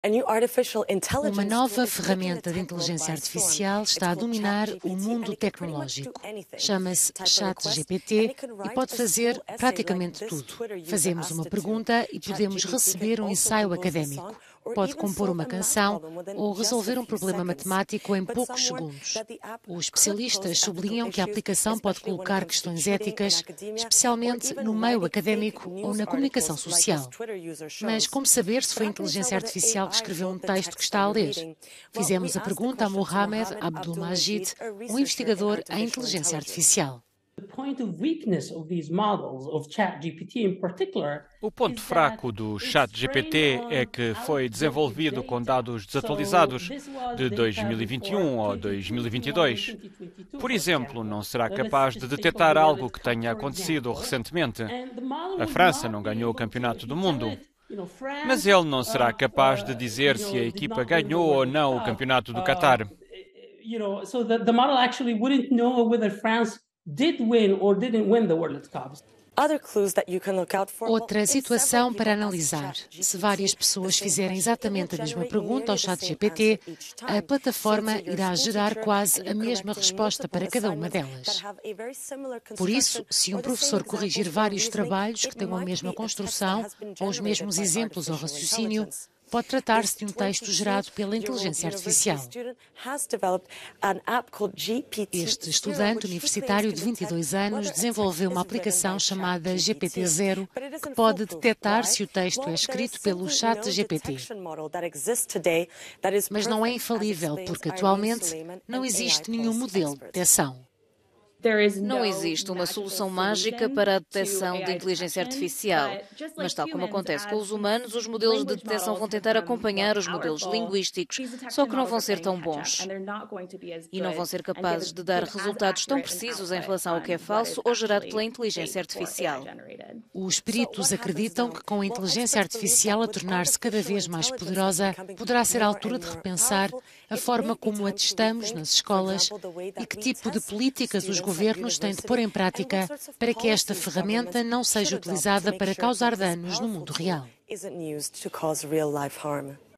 O "prodígio" da Inteligência Artificial
Plataforma ChatGPT criou uma revolução no mundo tecnológico